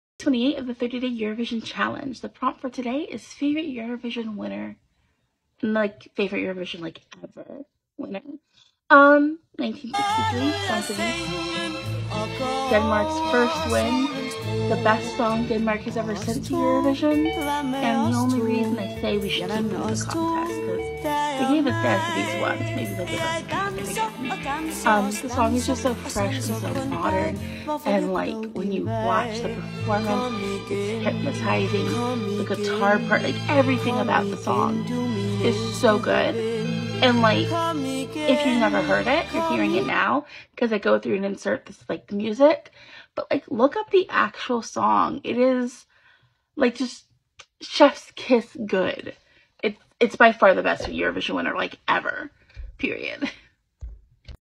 30 Day Eurovision Challenge 2023 - Day 28: Favorite Eurovision Winner | Lili Loves
Hello Everyone! ~♪

Continuing on with ESC_Shane's 30 Day Eurovision Challenge. Day 26 is "ESC2023 Song that describes you" and here is mine.

You can find the original card over on twitter at this

If you want more of me I am a part of the Eurovision Fam Team, check us out at: